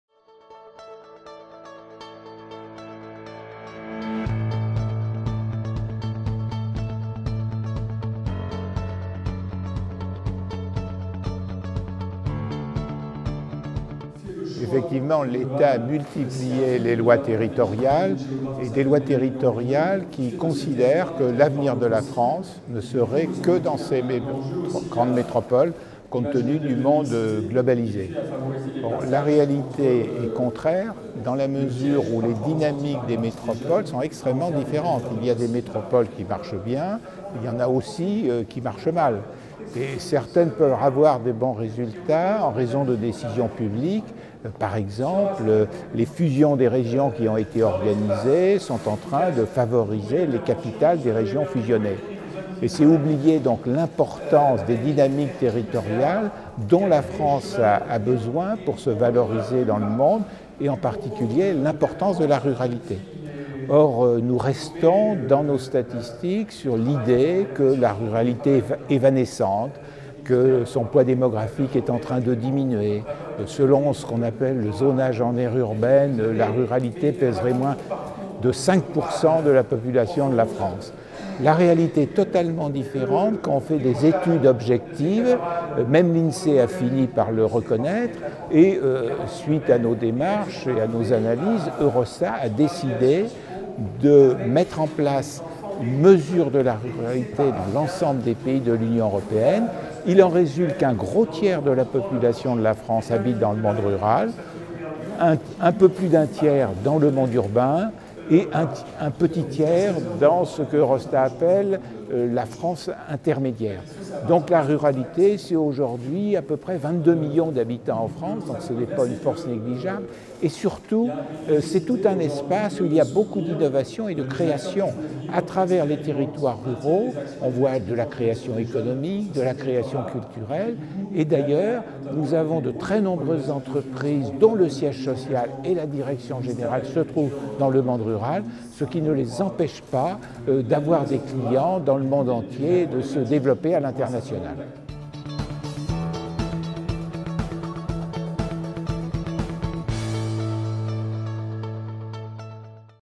Une question à Gérard-Francois Dumont
RURALISONS à la Bellevilloise le 4 juin 2019
Question à Gérard-François Dumont
Géographe

Image  Réjane Ruby
Montage  Eric Houdbine